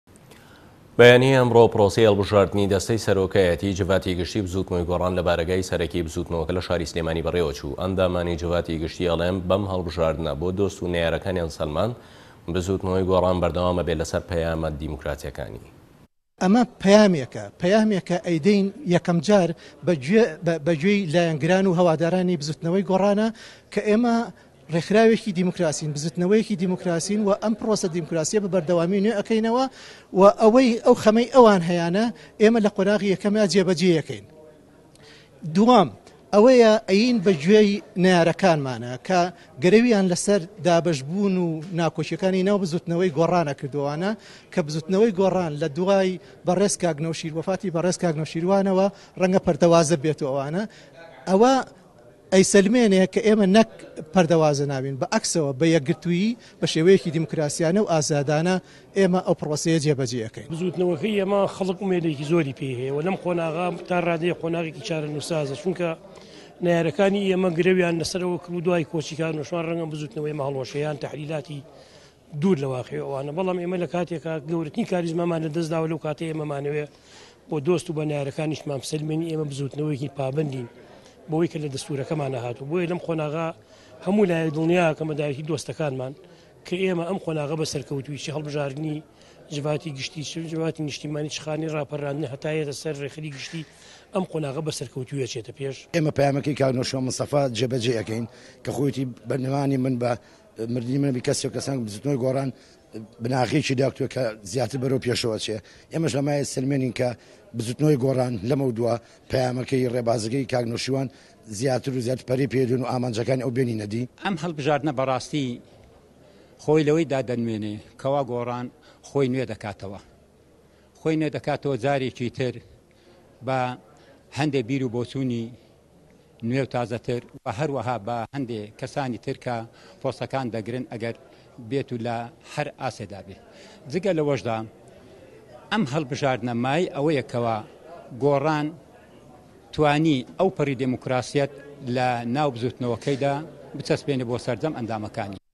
سلێمانی‌: بۆچونی‌ ئه‌ندامانی‌ جڤاتی‌ نیشتیمانی‌ گۆڕان له‌باره‌ی‌ داهاتووی‌ بزوتنه‌وه‌كه‌یانه‌وه‌
به‌یانی‌ ئه‌مڕۆ پرۆسه‌ی‌ هه‌ڵبژاردنی‌ ده‌سته‌ی‌ سه‌رۆكایه‌تی‌ جڤاتی‌ گشتی‌ بزوتنه‌وه‌ی‌ گۆڕان له‌باره‌گای‌...

به‌یانی‌ ئه‌مڕۆ پرۆسه‌ی‌ هه‌ڵبژاردنی‌ ده‌سته‌ی‌ سه‌رۆكایه‌تی‌ جڤاتی‌ گشتی‌ بزوتنه‌وه‌ی‌ گۆڕان له‌باره‌گای‌ سه‌ره‌كی‌ بزوتنه‌وه‌كه‌ له‌ شاری‌ سلێمانی‌ به‌ڕێوه‌چو، ئه‌ندامانی‌ جڤاتی‌ گشتی‌ ده‌ڵێن: به‌م هه‌ڵبژاردنه‌ بۆ دۆست و نه‌یاره‌كانیان سه‌لماند بزوتنه‌وه‌ی‌ گۆڕان به‌رده‌وام ده‌بێت له‌سه‌ر په‌یامه‌ دیموكراتییه‌كانی‌.